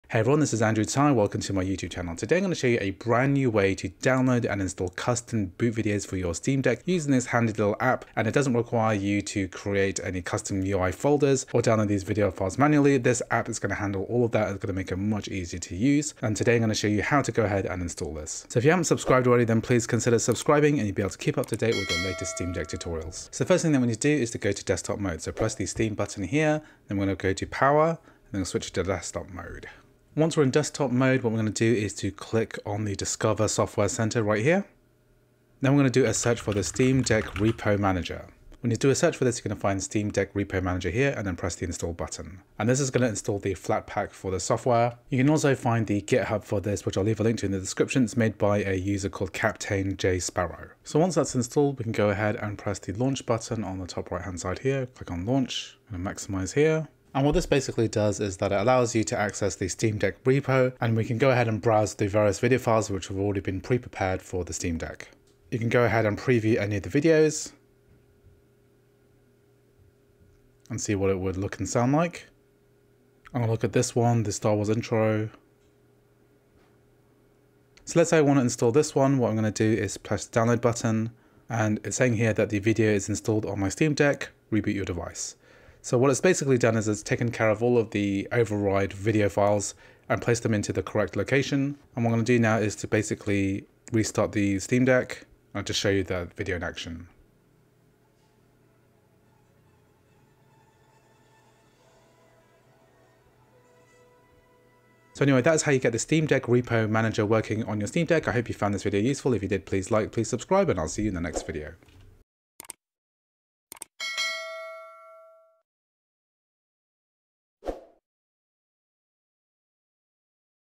NEW EASY GUI APP method for custom Steam Deck Boot animation - Steam Deck Repo Manager tutorial
► My Recording Setup:
►► My equipment: